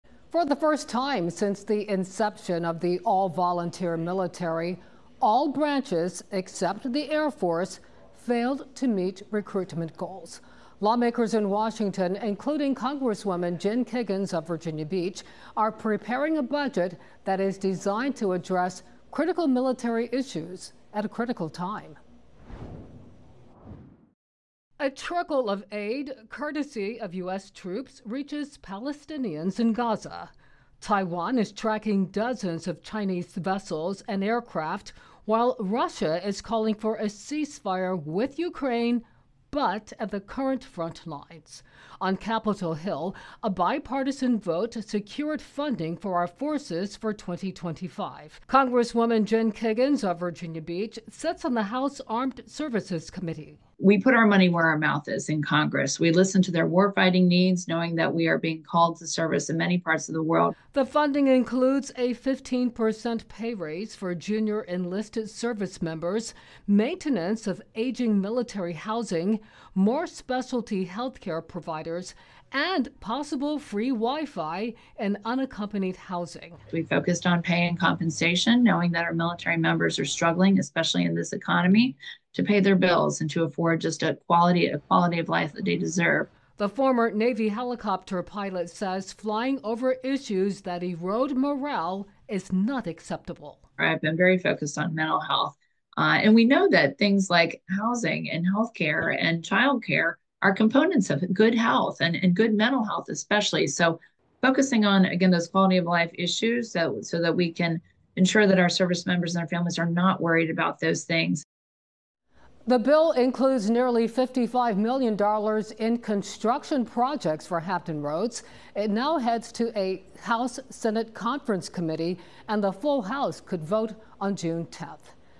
Kiggans praises passage of defense bill
Rep. Jen Kiggans praised the passage of the fiscal 2025 National Defense Authorization Act.